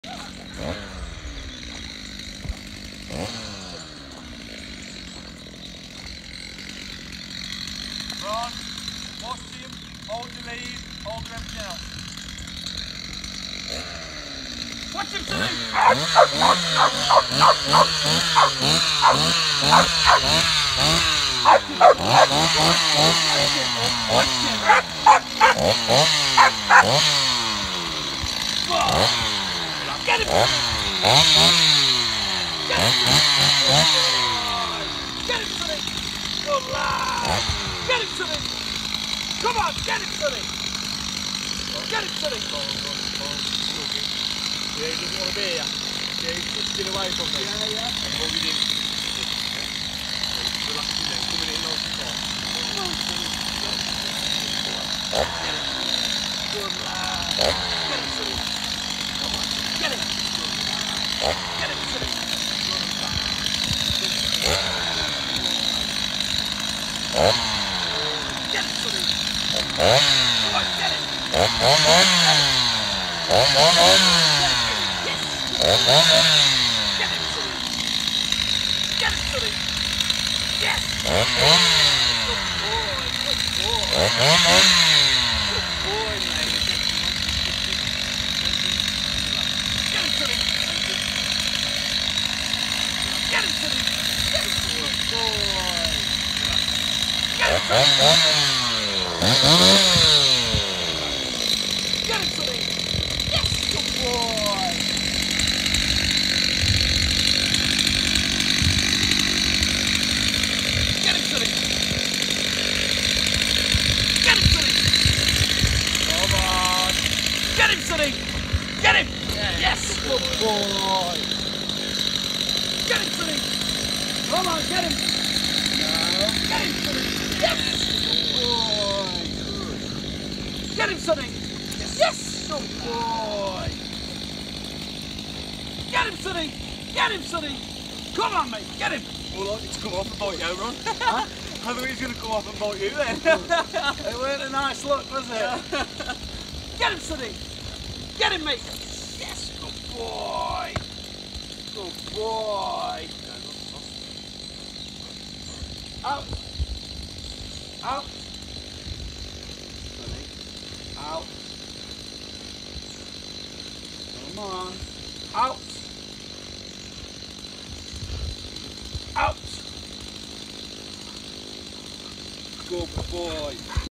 Environmental dog work with Asa Wright at Lorockmor working dogs
Amazing day testing what Sonny can do so we can best plan his training in the next 12.5 months

We also ran some security based stations to get ready for going back to work